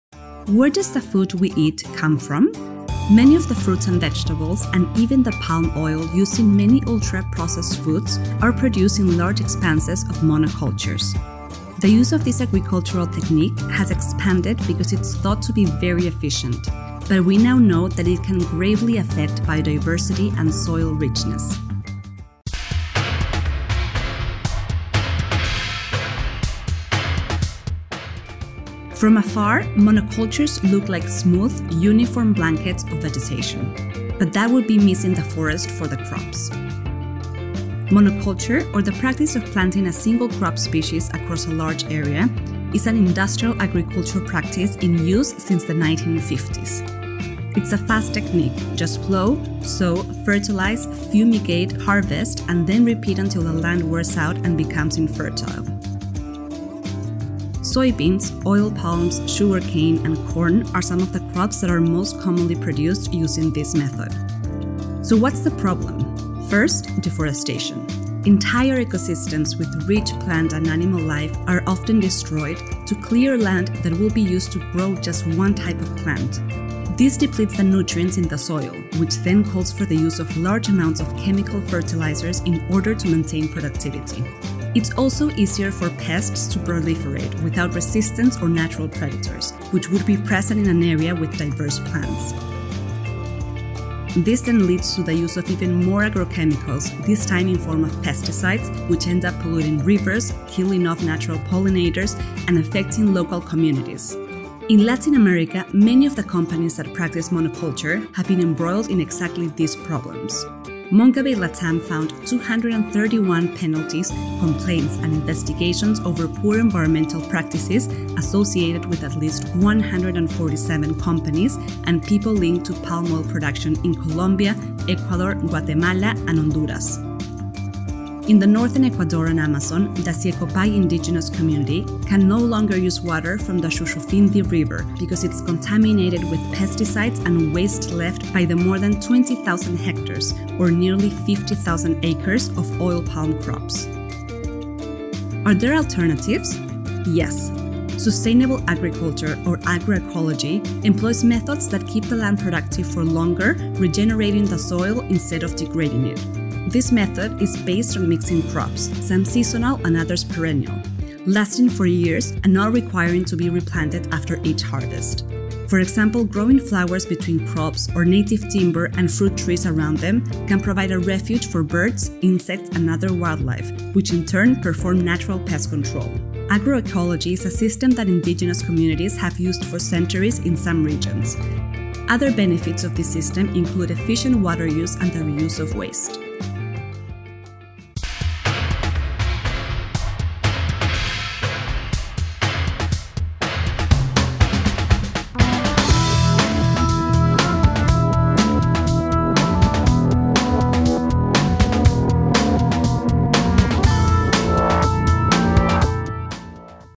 Why aren’t monoculture farms as valuable as the ecosystems they replace?
Many of the fruits and vegetables — and even the palm oil used in many ultra-processed foods — are produced in large expanses of monocultures. 2 The use of this agricultural technique has expanded because it’s thought to be very efficient. But we now know that it can gravely affect biodiversity and soil richness.

Are there alternatives? Yes. Sustainable agriculture, or agroecology, employs methods
that keep the land productive for longer, regenerating the soil instead of degrading it. Agroecology is a system that Indigenous communities have used for centuries in some regions. Other benefits of this system include efficient water use and the reuse of waste.